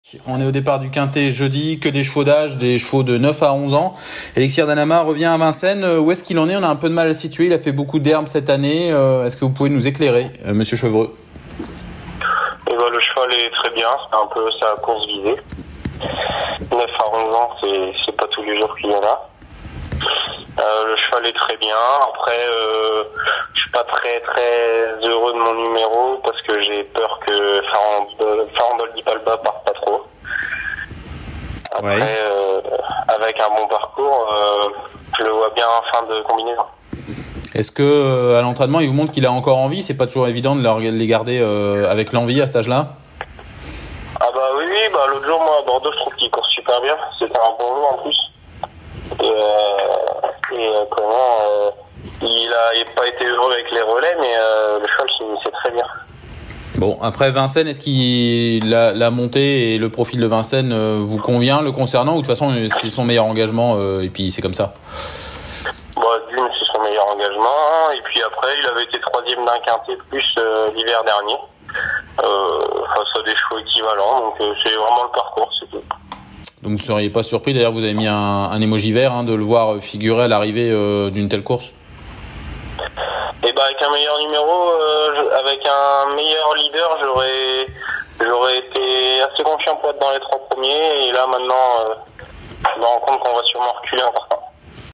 Julien Chevreux, entraîneur de Elixir d'Anama (Jeudi 18 janvier à Paris-Vincennes)
Julien Chevreux, entraîneur de Elixir d'Anama (11) nous parle de ses chances dans le Quinté+, le Prix d'Erbray sur l’hippodrome de Paris-Vincennes le Jeudi 18 janvier 2024.